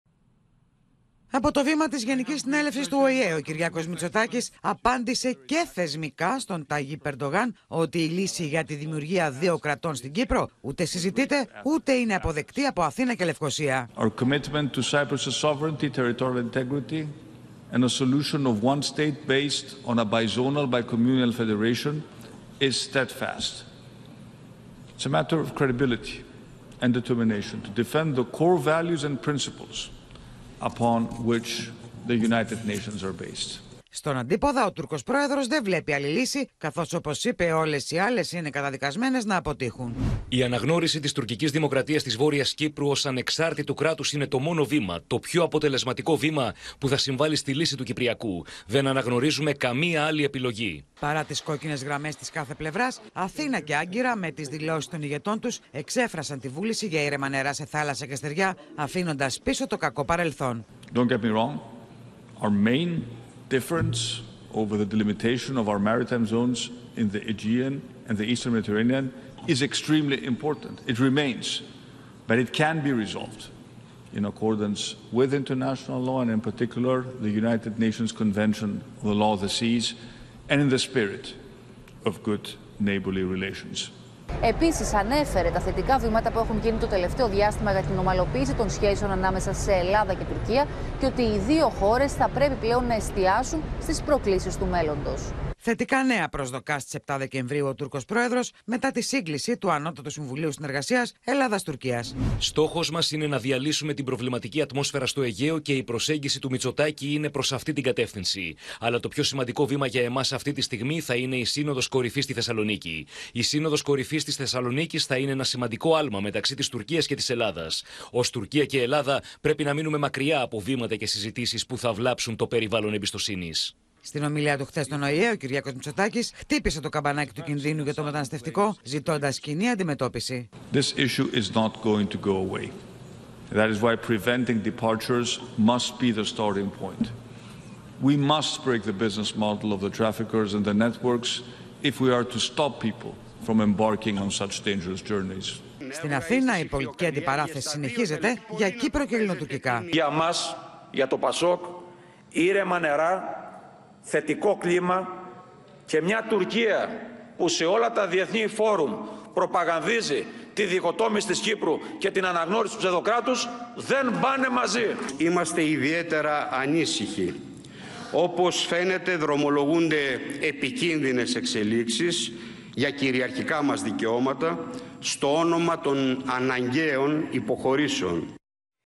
Μητσοτάκης στον ΟΗΕ: Ακλόνητη η θέση μας για ένα κράτος στην Κύπρο | OPEN TV
Ο Κυριάκος Μητσοτάκης επανέλαβε από το βήμα του ΟΗΕ τις κόκκινες γραμμές της Αθήνας και την μόνη διαφορά με την Άγκυρα σχετικά με την οριοθέτηση των θαλασσίων ζωνών σε Αιγαίο και Ανατολική Μεσόγειο. Παράλληλα υπογράμμισε την ακλόνητη δέσμευση της Ελλάδας για την λύση ενός κράτους, την δέσμευση για την κυριαρχία της Κύπρου και την εδαφική της ακεραιότητα. Ο Ταγίπ Ερντογάν επιμένει στην προκλητική θέση της αναγνώρισης του ψευδοκράτους λέγοντας πως δεν υπάρχει άλλη λύση.